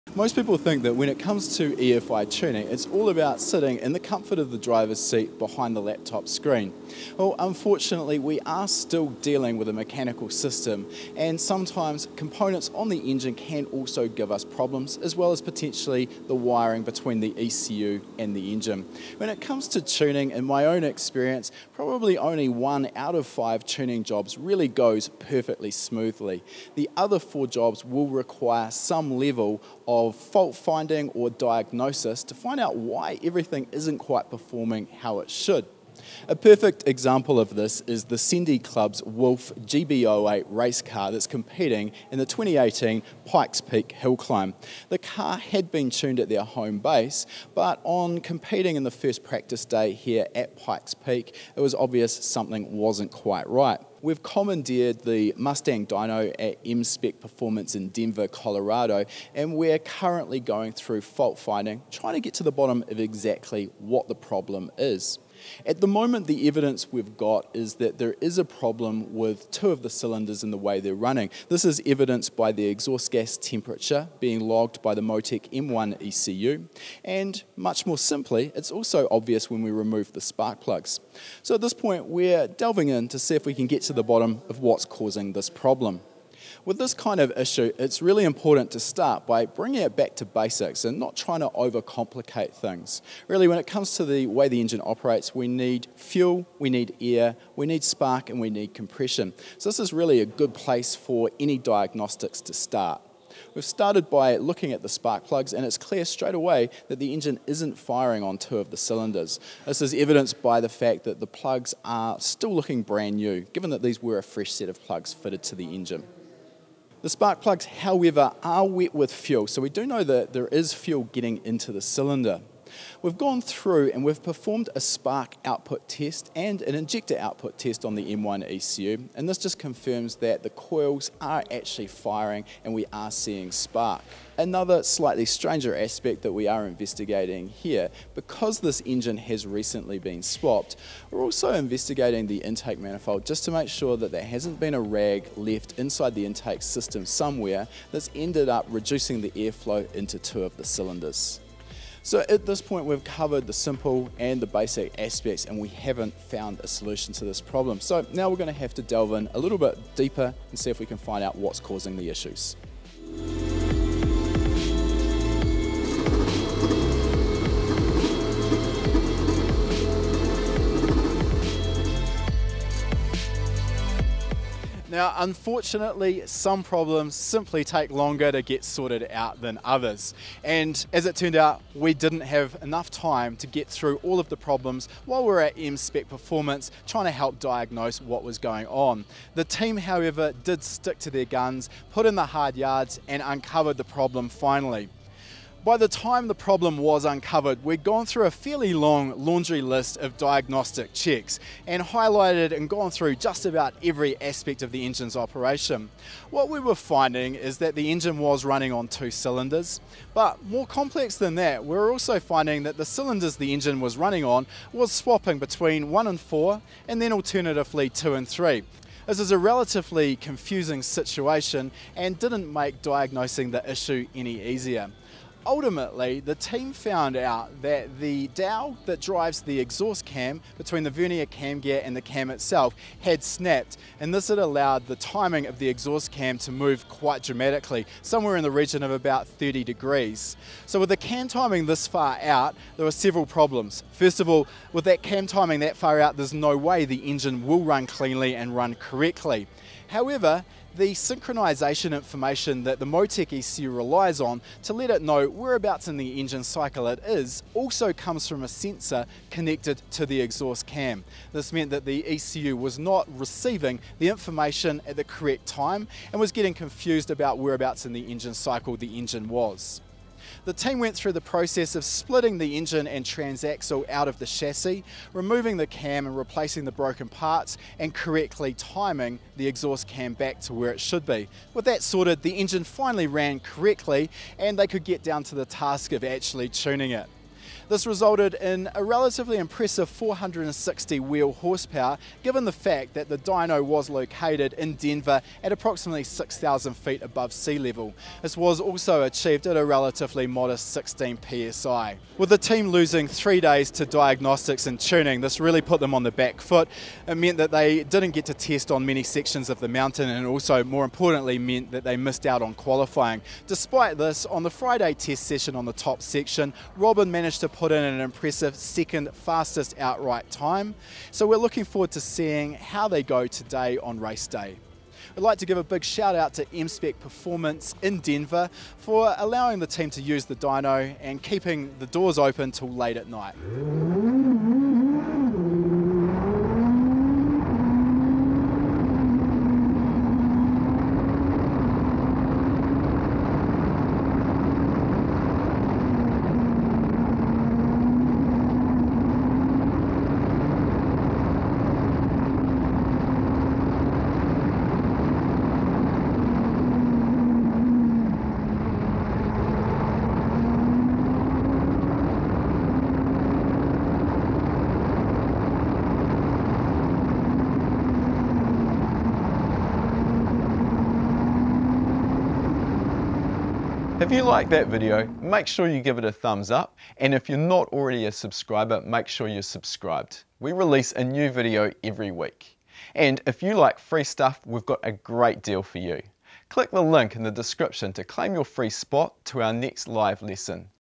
Can you guess what was wrong? We couldn't.
A 550kg vs 550hp car is great when running correctly, but in the world of EFI tuning and motorsport, not everything always goes to plan.

At the 2018 Pikes Peak International Hill Climb (Official Site) we were lucky enough to spend some time with Robin Shute Racing's 2018 Wolf Racing Cars TSC Honda K20 powered car, but not in the circumstances we would have liked.

In our own experience 1 out of 5 tuning jobs will go perfectly leaving 4 that require varying degrees of fault finding, diagnosis and troubleshooting ranging from simple things such as an empty fuel tank, oversights like the wrong spark plug after an FI conversion, to electrical gremlins on recently installed ECUs, and of course much worse is also possible.

With this in mind having a set process you can work through to methodically work through and find the source of a problem is a handy skill set to have. In this video, Andre runs us through some of what the team did and what the ultimate issue was that unfortunately caused them to miss out on qualifying for the event. While it was a disappointment for the team to fall at almost the last hurdle, the experience will have certainly helped grow the knowledge base the team shares making them stronger for their return next year.

Want to learn how to tune EFI cars like this? Start here for free